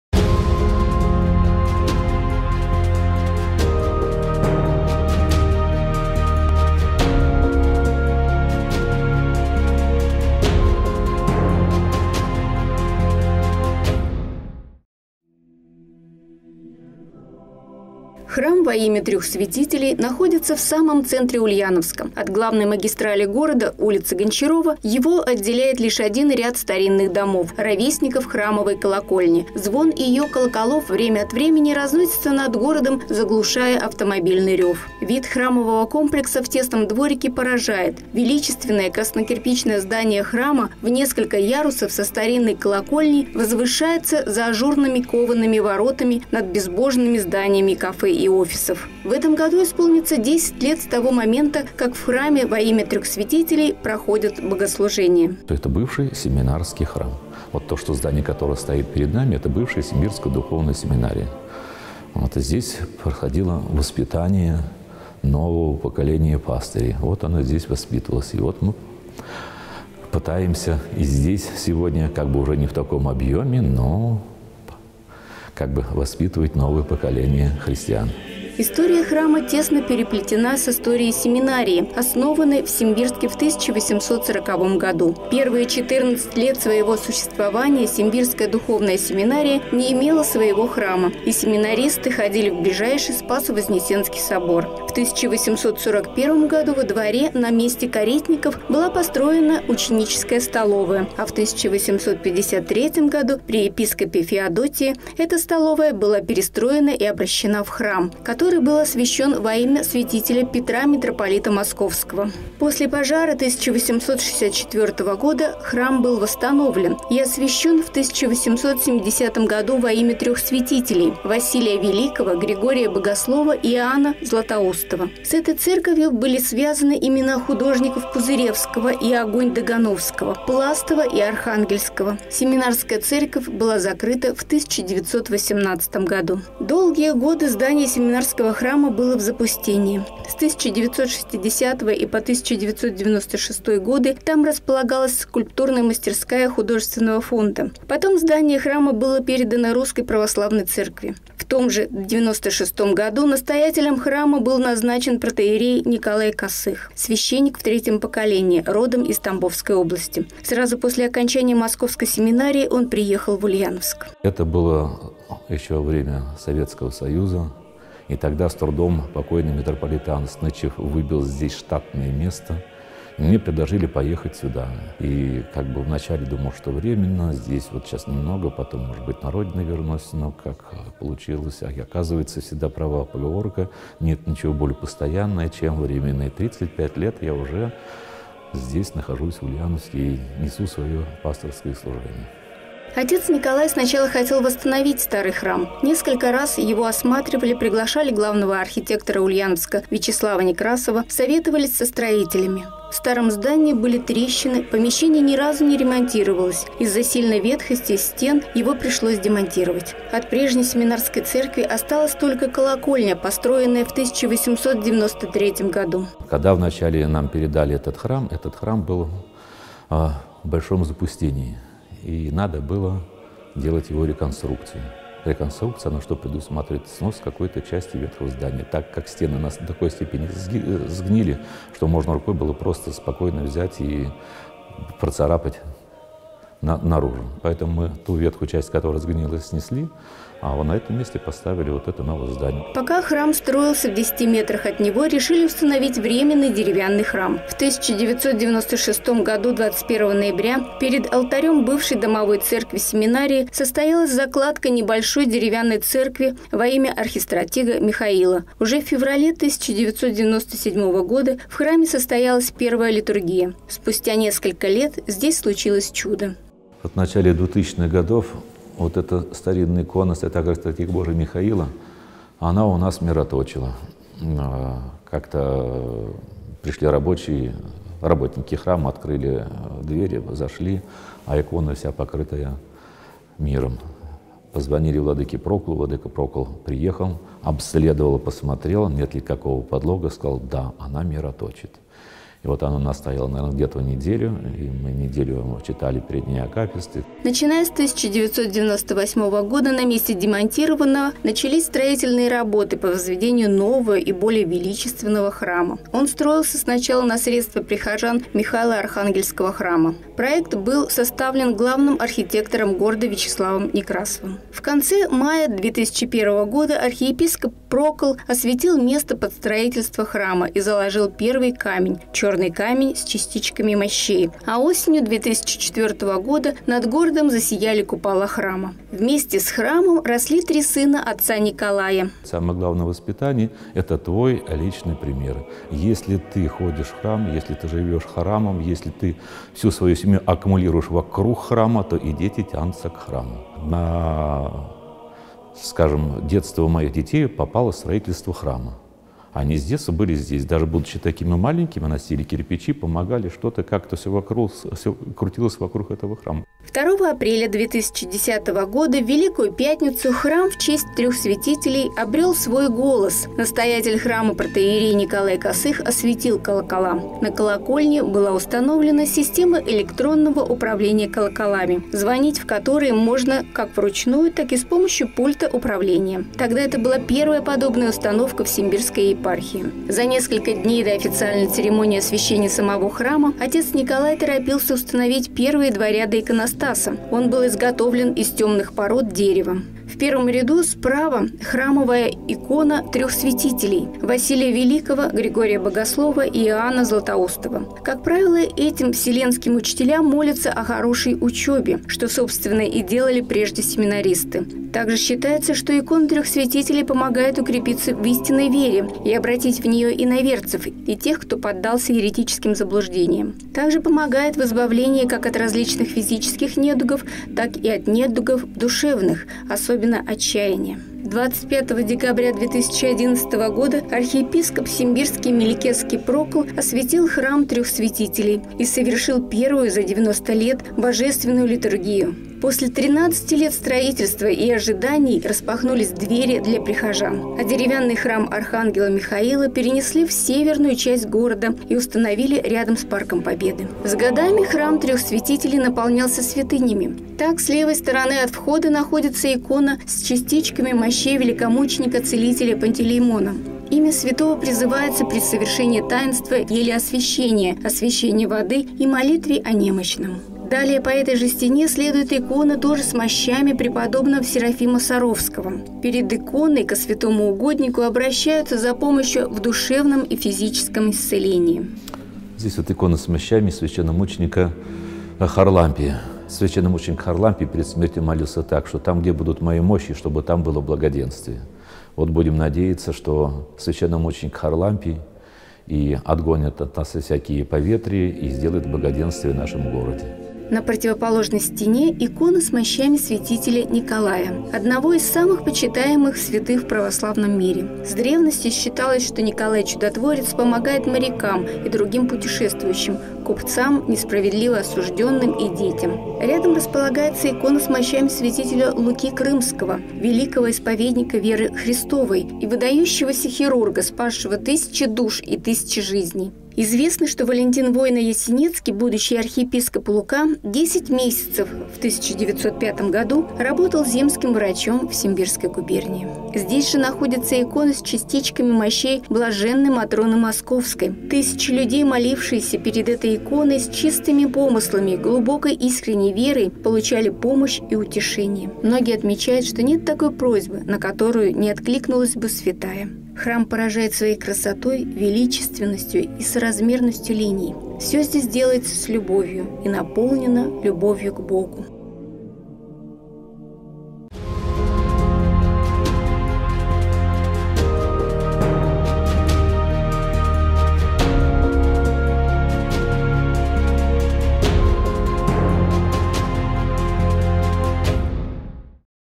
Святыни земли Симбирской. Какие тайны хранит храм во имя Трёх Святителей
От главной магистрали Ульяновска, улицы Гончарова, храм отделяет лишь один ряд старинных домов – ровесников храмовой колокольни. Звон её колоколов время от времени разносится над городом, заглушая автомобильный рёв.